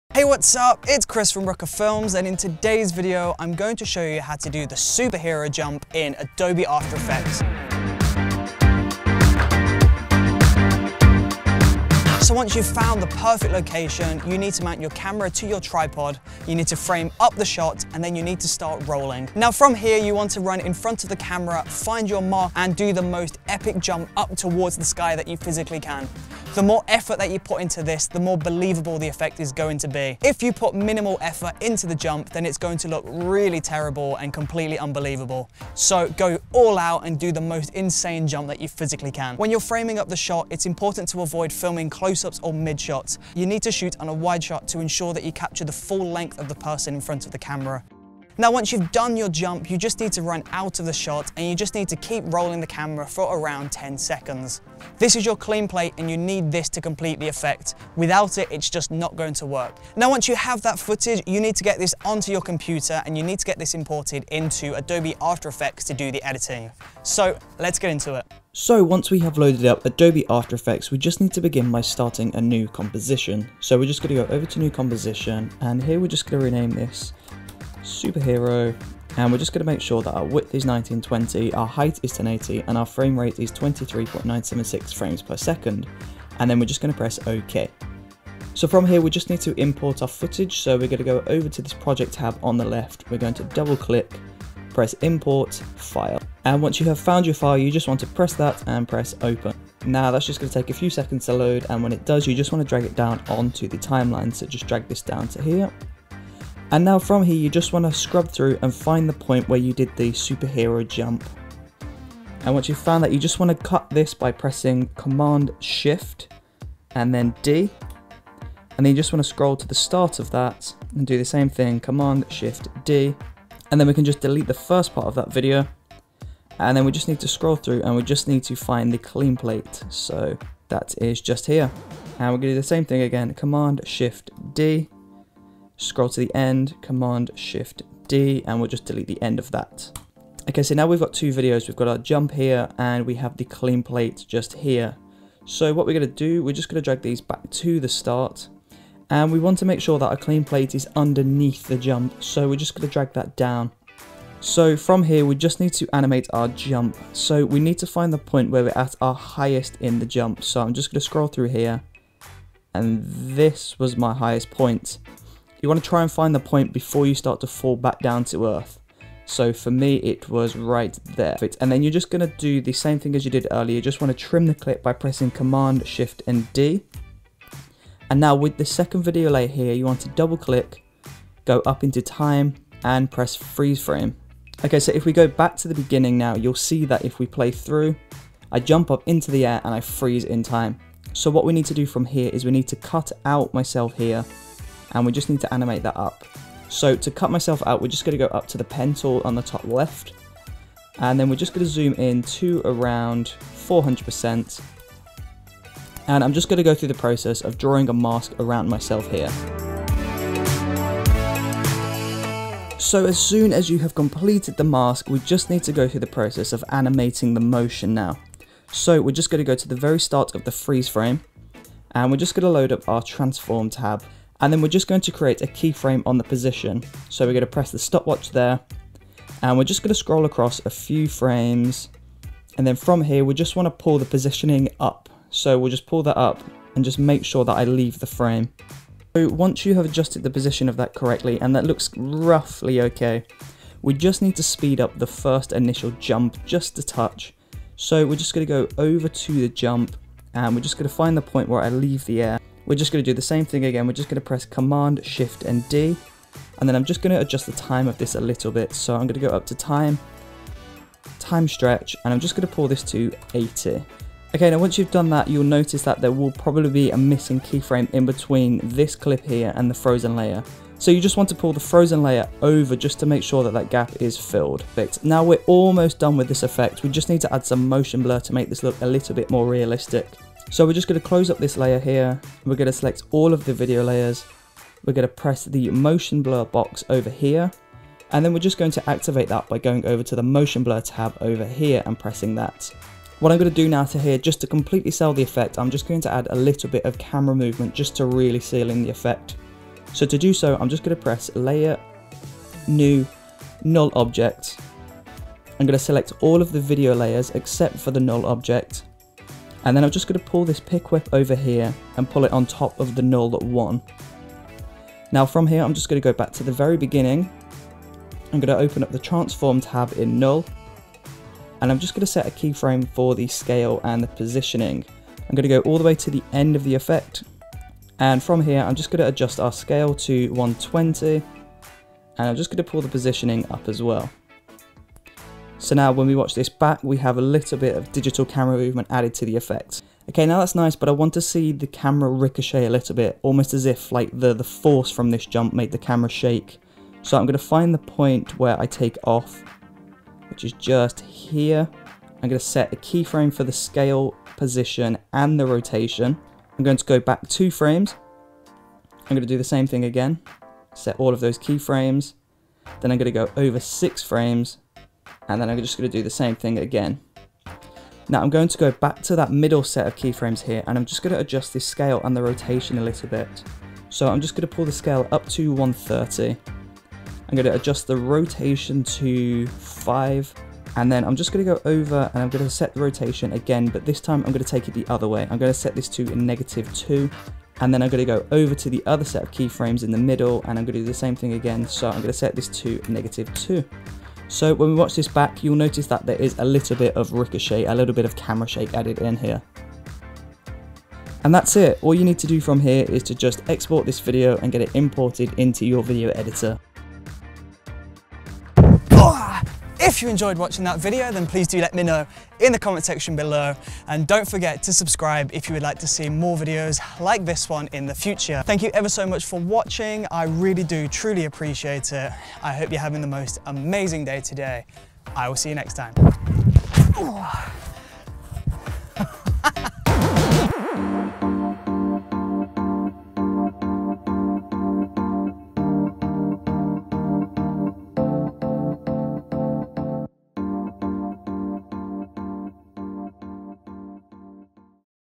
Jump Like A SUPER HERO! (After Effects Tutorial)
Jump like a super hero with a camera, a tripod and Adobe After Effects!